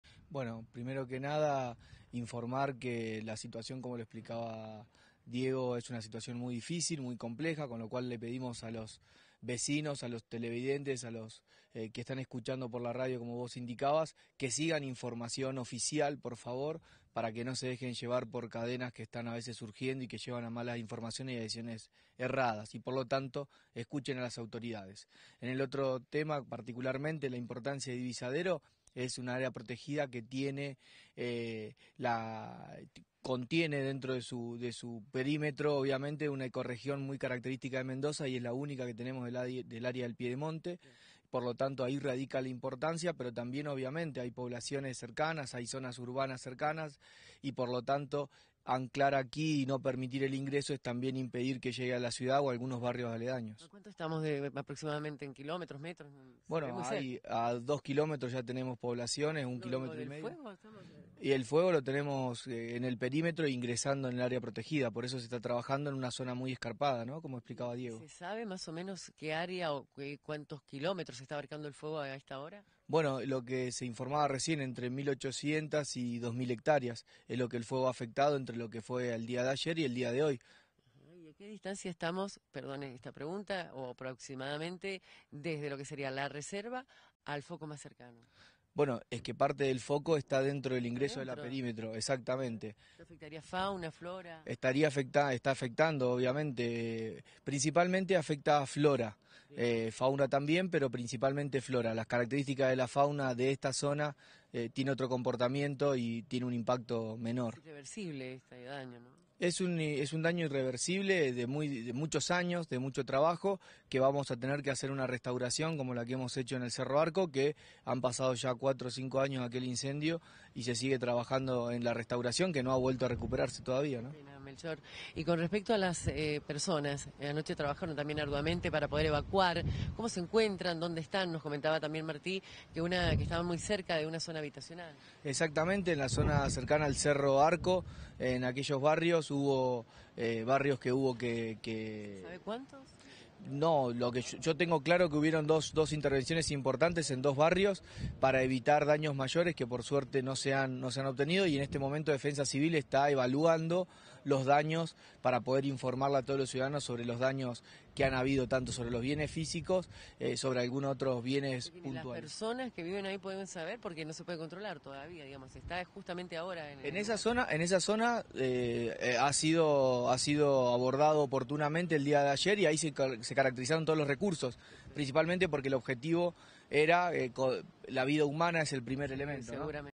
➡️OTRA ZONA AFECTADA
-En la mañana de este domingo informaron que las afectaciones fueron en gran parte de campo y en algunas viviendas, a las cuales evalúan las condiciones de habitabilidad.

-Otra parte afectada es en la reserva Divisadero Largo, en el sector Oeste alejado del circuito para visitantes.